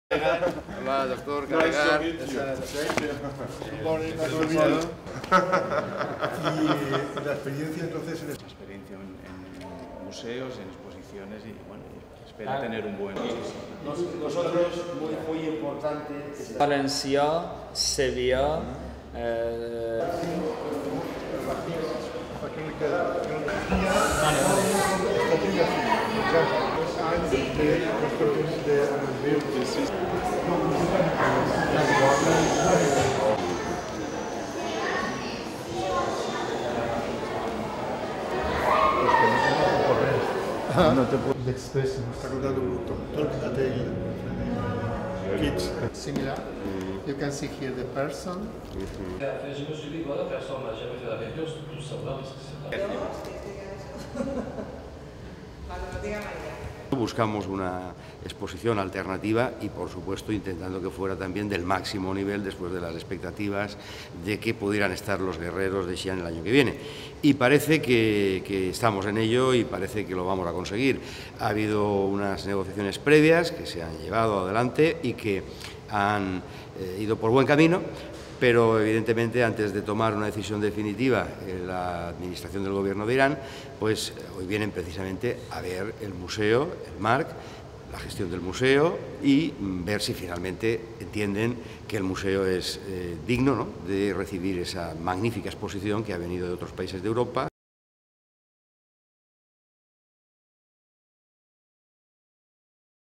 319 Visita del director del Museo Nacional Irán
El diputado de Cultura, César Augusto Asencio, ha recibido esta mañana en el MARQ al director general de los Museos Nacionales de Irán, Mohammad Reza Karegar, y al consejero cultural de la Embajada de Irán, Alireza Esmaeili, para mostrarles in situ las instalaciones del museo y recibir el visto bueno para acoger la exposición internacional ‘Irán: Cuna de Civilizaciones’. La delegación iraní ha recorrido las distintas salas del centro donde han podido admirar las muestras permanentes, la última propuesta expositiva que profundiza en el Arte Rupestre, así como la zona del sótano donde se encuentra el Gabinete de Investigación y el Laboratorio de Restauración, entre otras.
El próximo proyecto internacional del MARQ cuenta con 194 piezas de alto valor, veinte de ellas de oro, e incluye la impactante réplica del ‘Friso de los arqueros’ de Darío el Grande (510 a.C.) que estará acompañado de dos grandes esculturas originales. La exposición se podrá visitar entre marzo y agosto y hará un recorrido por la historia de Irán, desde la primera presencia humana en la zona, hace unos 300.000 años, hasta la islamización, en el siglo VII de nuestra Era.
 La comitiva se ha completado, además, por el director gerente de la Fundación MARQ, José Alberto Cortés, el director técnico del MARQ, Manuel Olcina, el jefe de Exposicionesy el jefe de Colecciones del museo, Jorge Soler y Rafael Azuar, respectivamente, y el director del Seminario Permanente de Iranología de la Universidad de Alicante, José Cutillas.